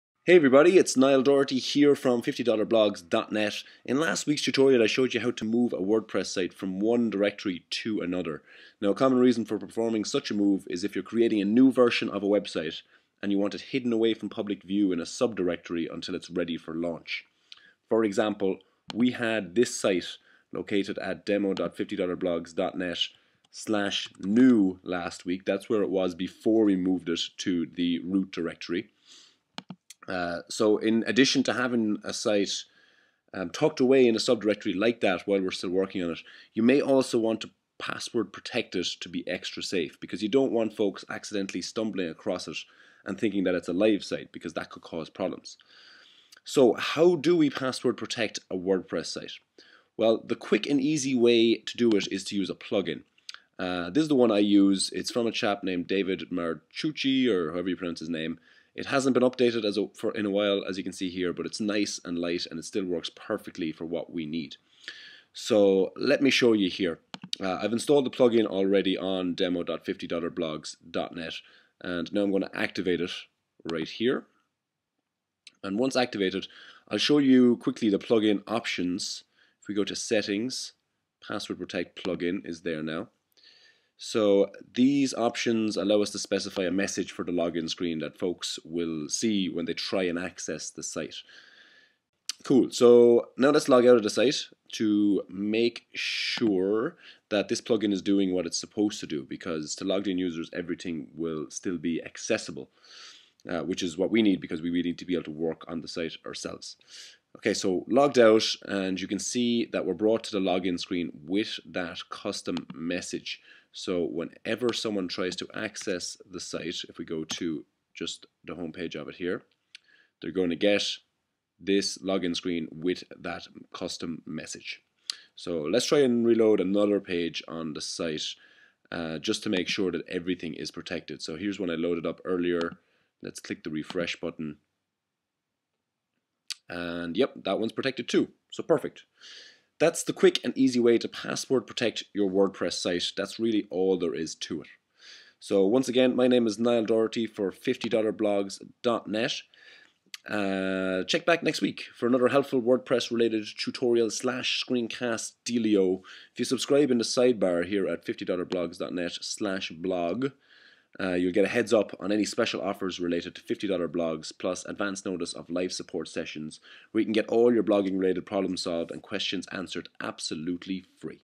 How To Password Protect An Entire WordPress Site - $50 Blogs
Highlights from the video:

[0:05] A common reason for password protecting your WordPress site
[0:46] The quick and easy solution: Password Protect Plugin for WordPress
[1:07] Activating and configuring the plugin
[1:32] Testing the plugin, making sure it works as expected